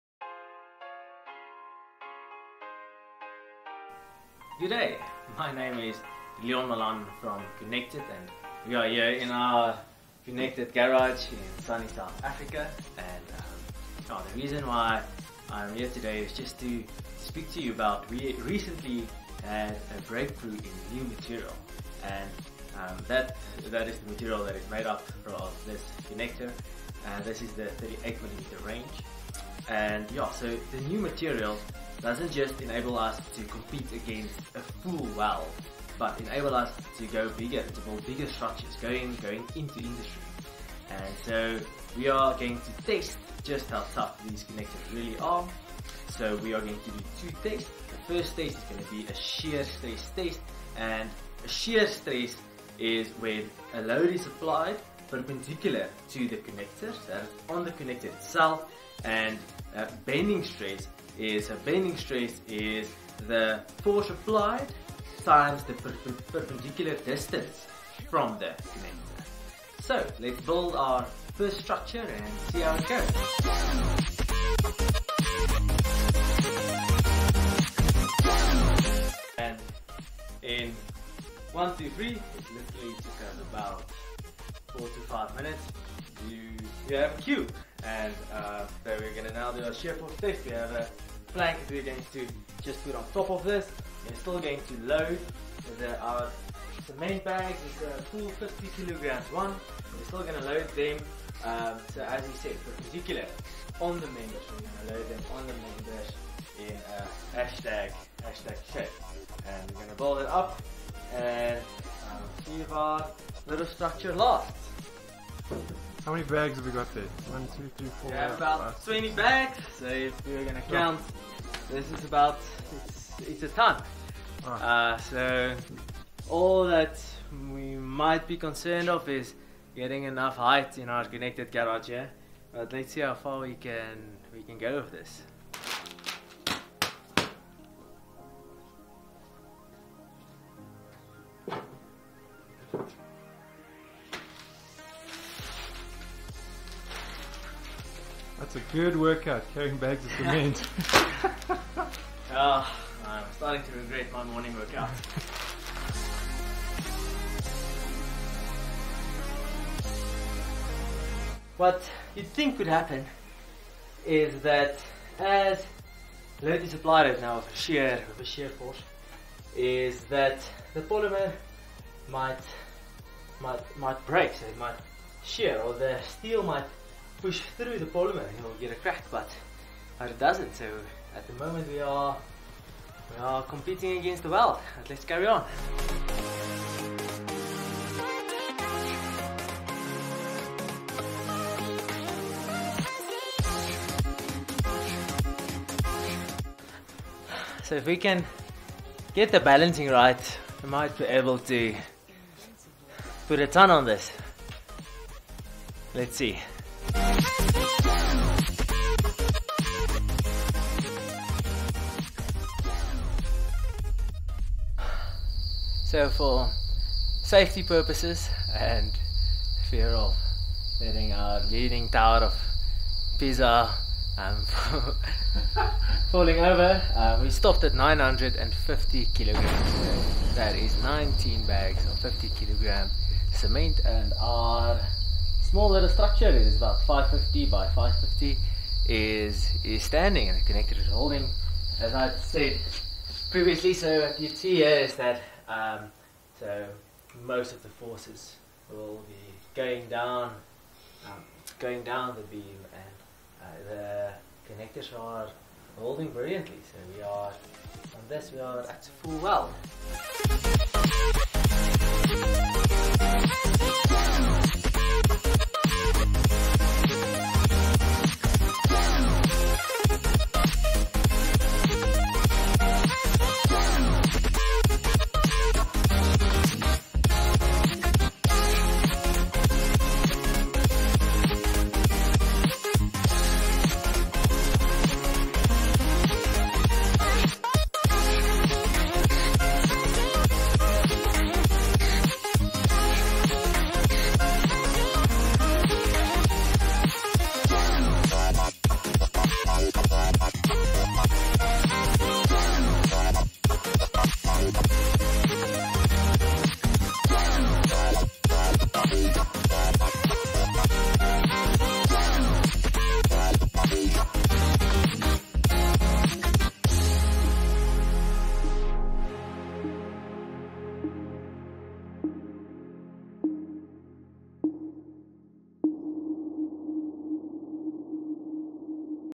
Testing the new 38mm Connector with bags of cement!
The future of Connect-it is S T R O N G !!!
Can you guess how many bags of 50kg cement our 550x550mm cube made of 38mm square connectors and steel can hold?